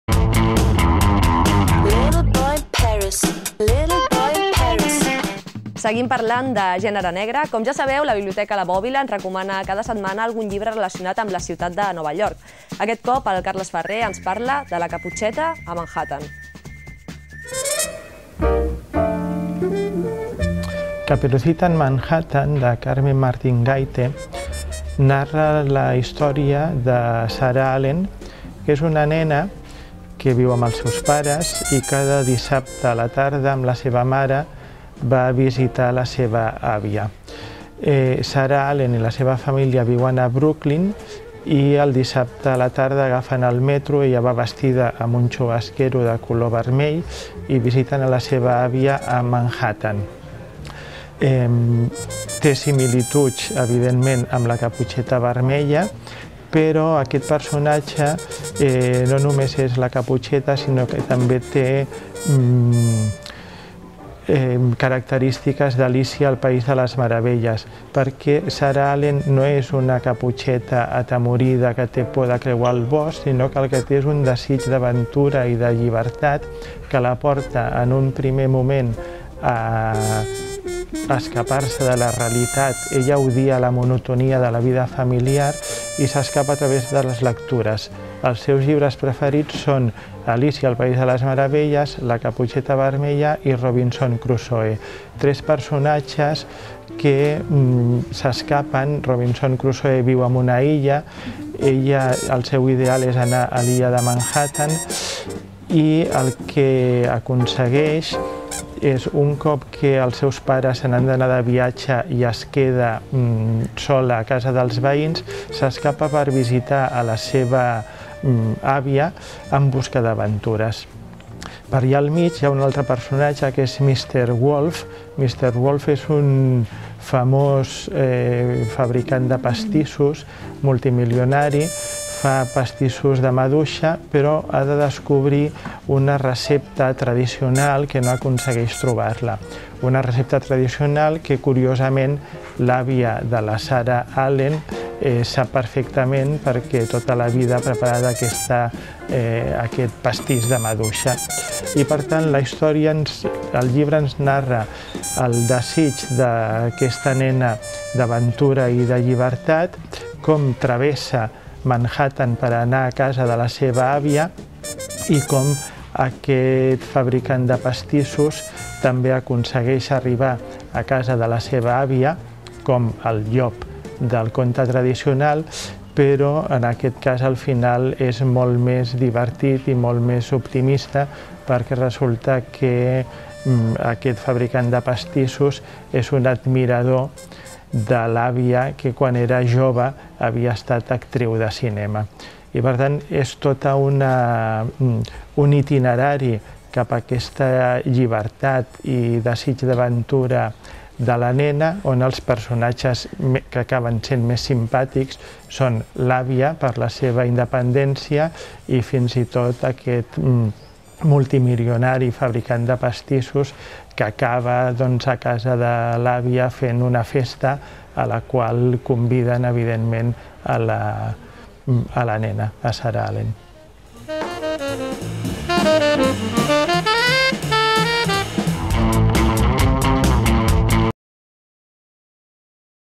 imperdible49
Presentació de la novel·la "Caperucita en Manhattan" de Carmen Martín Gaite, a càrrec de Carles Ferrer, de la Biblioteca la Bòbila